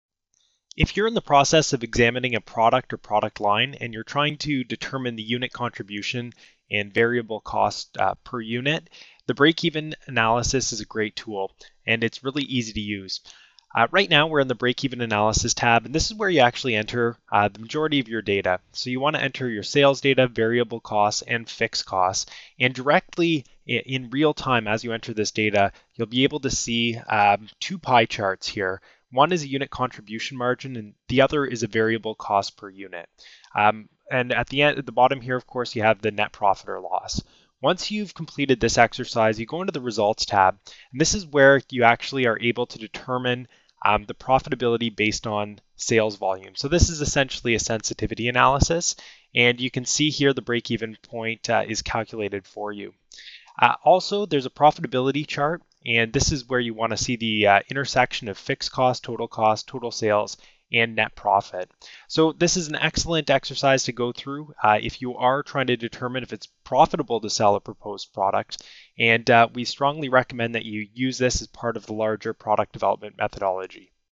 Break Even Analysis Template
Use Demand Metric's Break Even Analysis Template to input cost & sales variables and to determine a break even point and profitability.